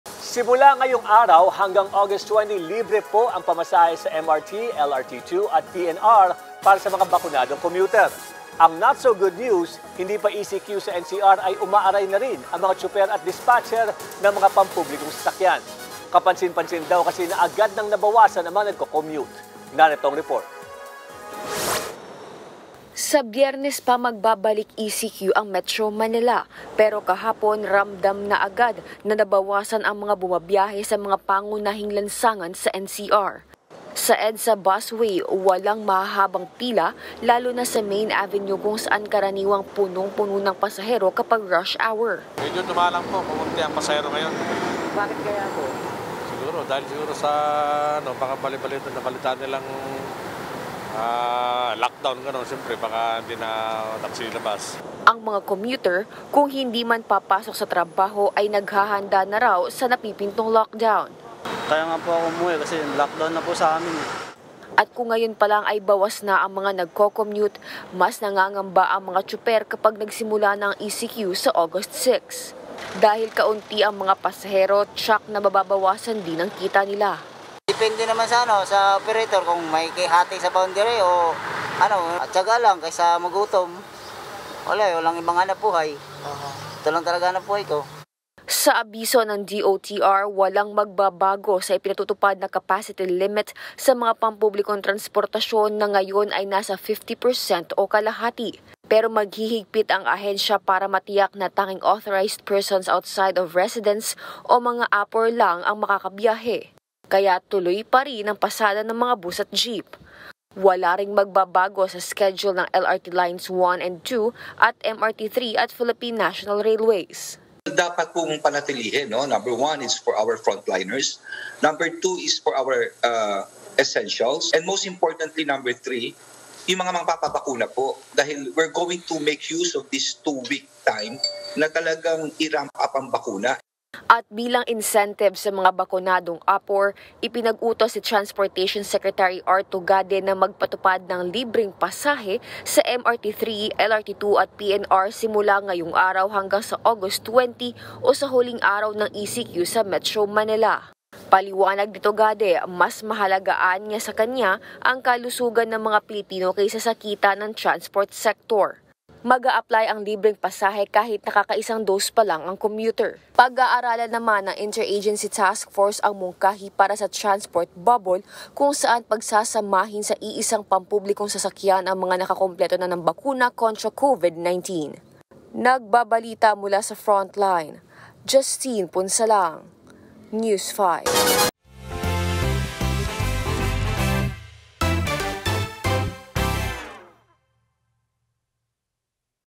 Mga tsuper at dispatcher, umaaray sa maliit na kita bago mag-ECQ
FrontlineSaUmaga | Nangangamba ang mga tsuper at dispatcher ng mga pampublikong sasakyan sa tiyak na bawas kita dahil sa kakaunting pasaherong bumabiyahe ilang araw bago ipatupad ang ECQ sa NCR. | via Justinne Punsalang